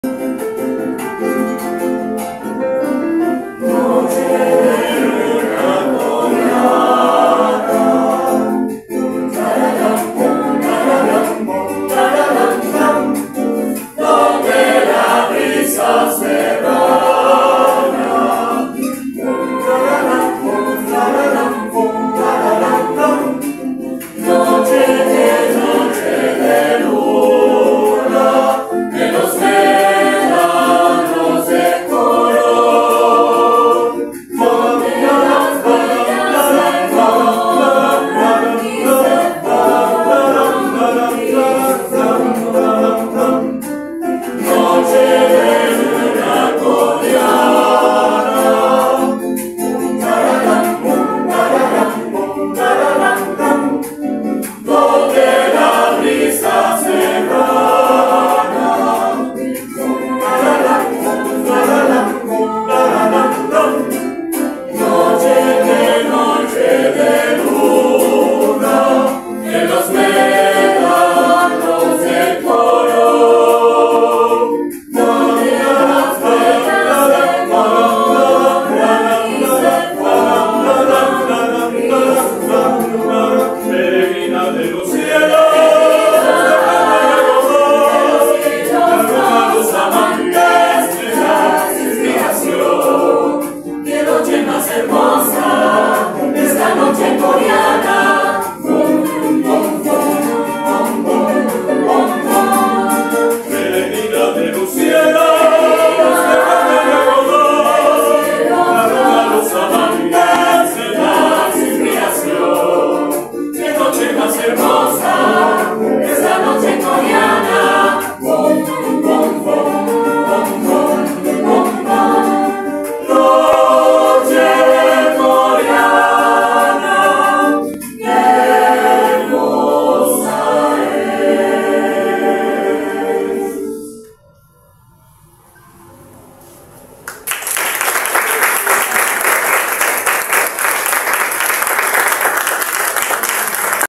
🌟 EnVideo | Presentación Inaugural de la Coral UNICA, dirigida por el Prof. Ricardo Chourio.
🔶 Felicitamos de manera fraterna a nuestra comunidad estudiantil que integra la Coral Universitaria, agradeciendo igualmente a nuestros docentes del Programa de Música por el apoyo a esta iniciativa.

🌟 Somos
¡Excelencia Académica y Compromiso Social!